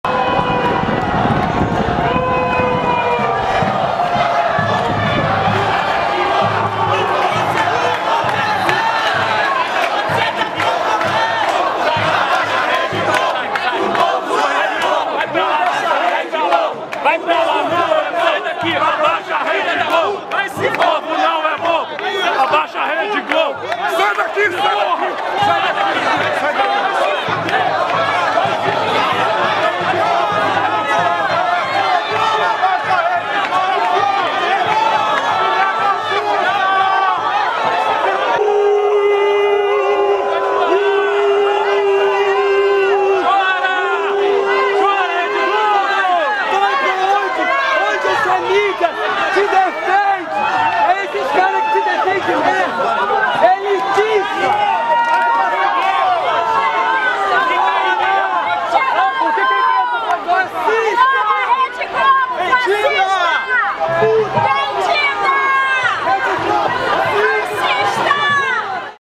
Educadores expulsam Rede Globo de ato no RJ
Educadores expulsam Rede Globo de ato no RJ,  em repudio a matérias de ataques aos trabalhadores da educação e sua luta legitima por melhores condições de trabalho e educação pública de qualidade.